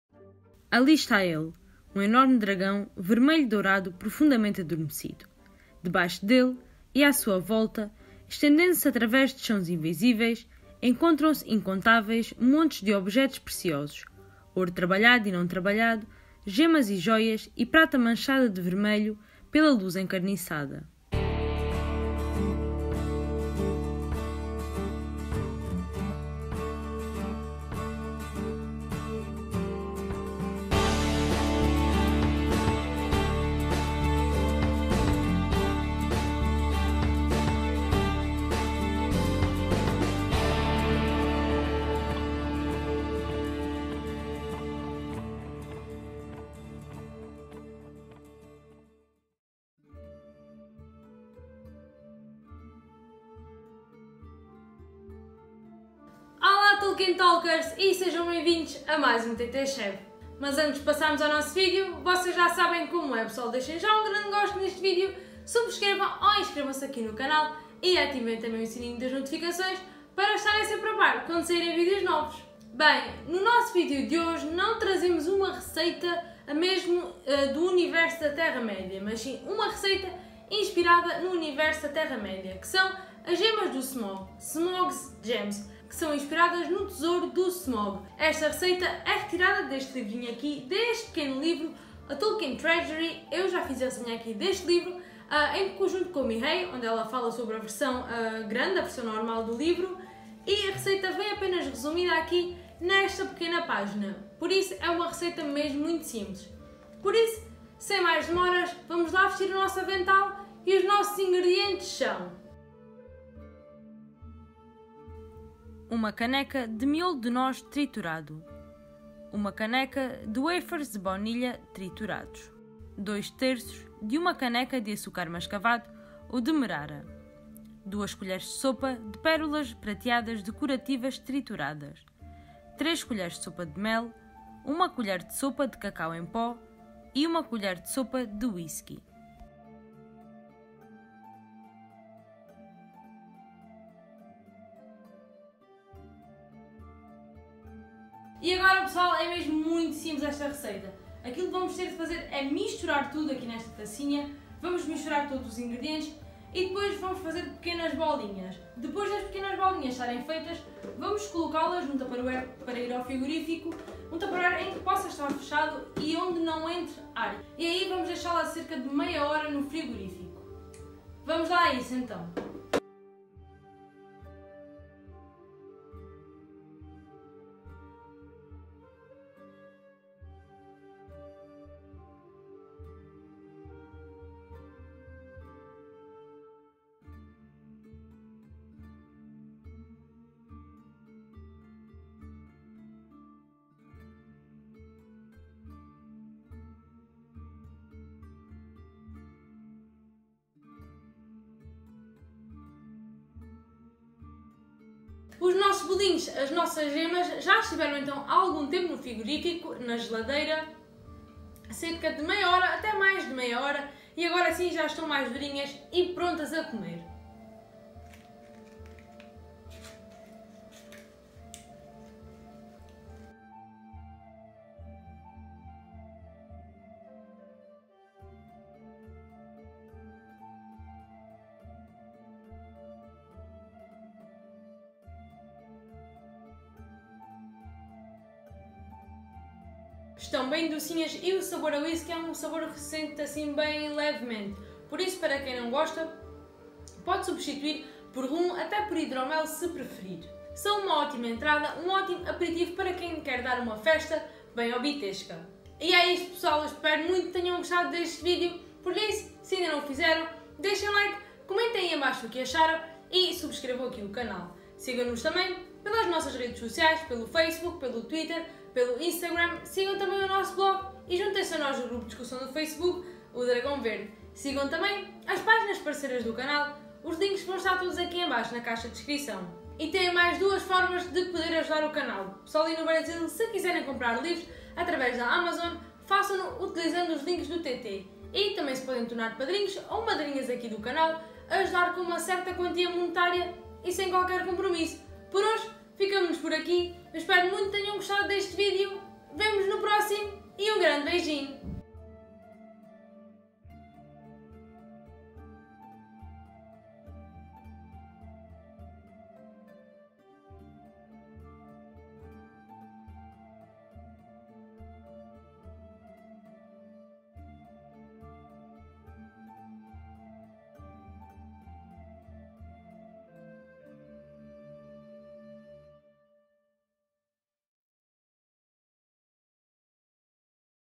Cozinha de O Hobbit: Gemas do Smaug | TT Chef 13
Smaug deitava-se num leito de jóias, gemas e pedras preciosas. Inspiradas no seu tesouro roubado aos anãos, as Gemas do Smaug são um aperitivo doce muito fácil de confecionar que irá animar qualquer banquete hobbitesco.

IMAGENS:

Cory Godbey, Justin Gerard